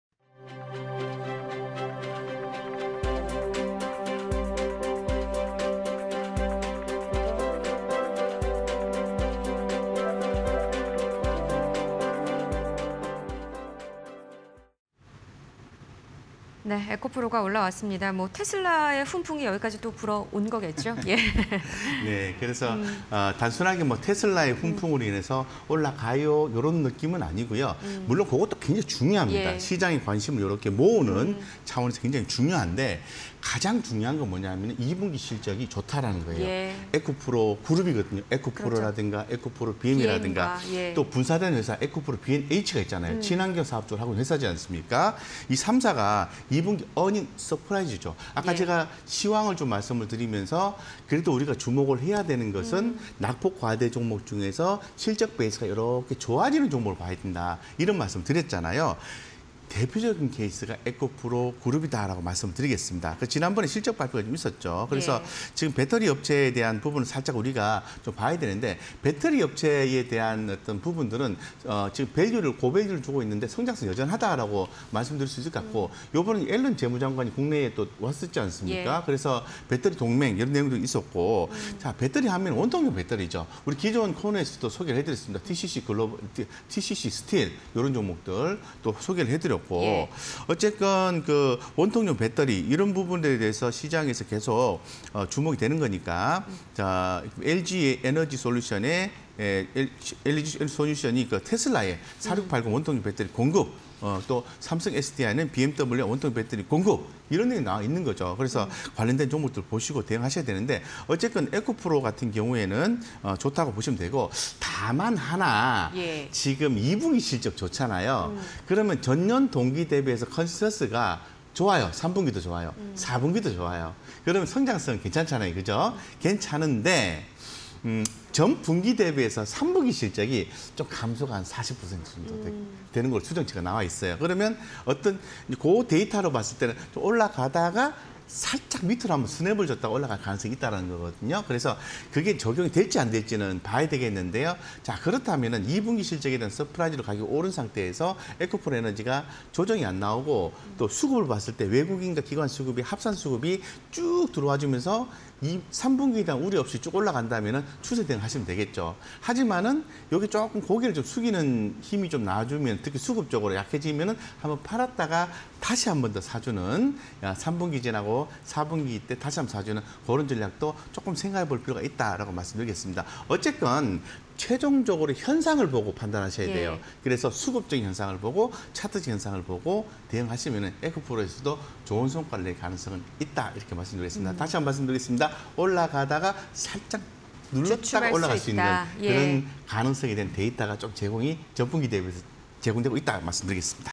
에코프로(086520)... 에코프로 그룹주, 호실적 등에 상승_테마IN이슈 (20220722)
에코프로(086520)
- 에코프로 그룹주, 호실적 등에 상승
- 'K-배터리' 업체, 글로벌 시장 공략 중
- 에코프로 그룹주·삼성SDI 등 2차전지주 상승